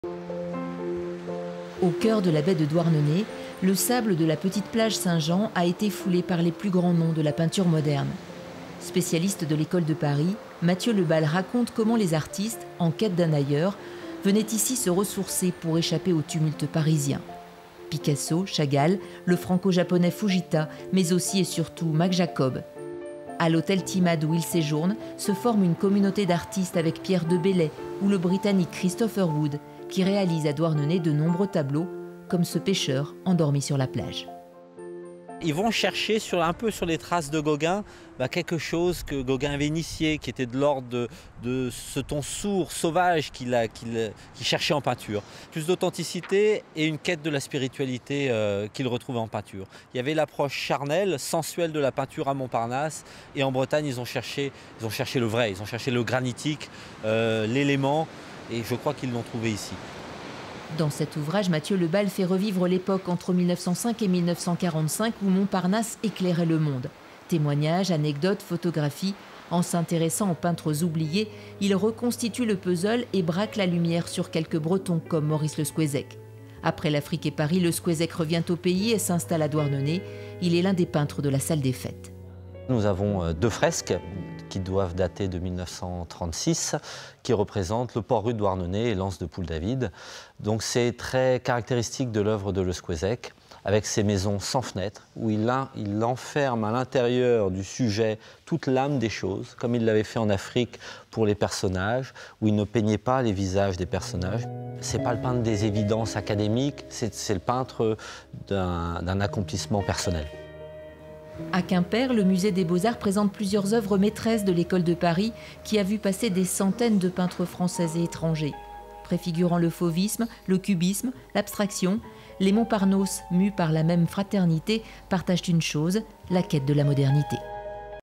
Montparnasse, quand Paris éclairait le monde, Mathyeu Le Bal, Albin Michel, 2022
Reportage sur France 3 Bretagne du 17 janvier 2023. A l'occasion de la parution du livre de Mathyeu Le Bal, Montparnasse, quand Paris éclairait le monde chez Albin Michel. Reportage mettant en lumière le lien à la fois historique et artistique entre la Bretagne et le quartier parisien de l'art moderne. Réalisation C.Louet.